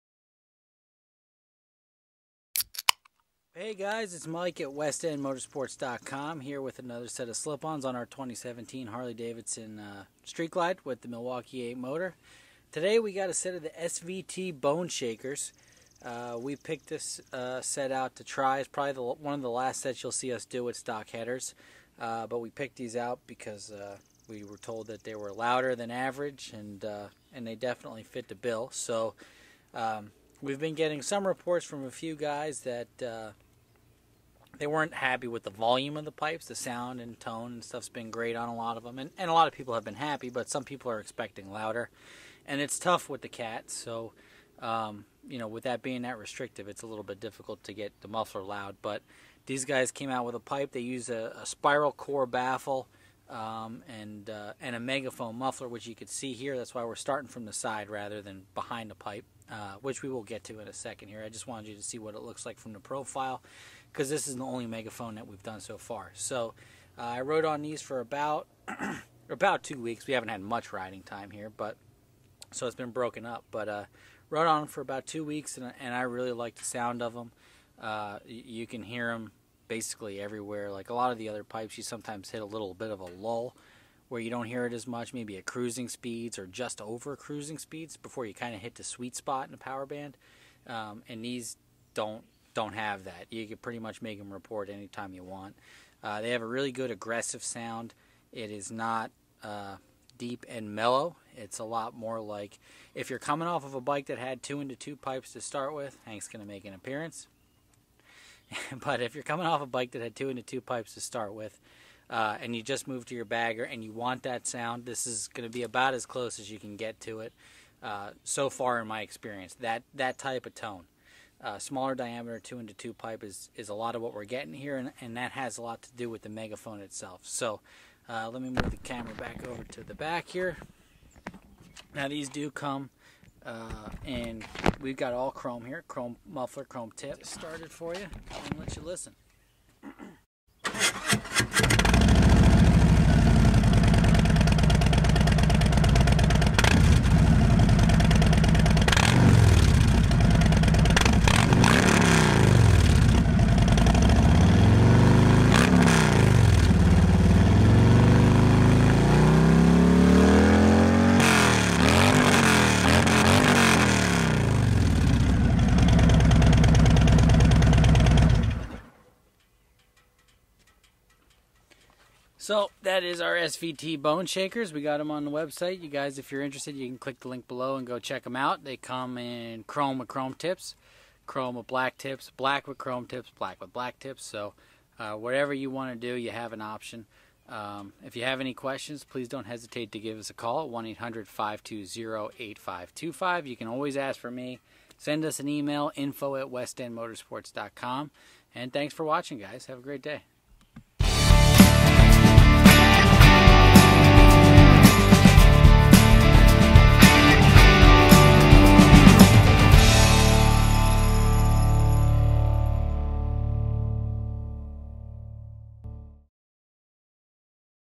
SVT Boneshakers - 2017 Harley Davidson Touring Sound & Review
Check out the sound on these new SVT Boneshakers on our 2017 Harley Davidson with Milwaukee 8 motor. By far the most aggressive slip on we've used with stock headers, so if you want loud - you got it!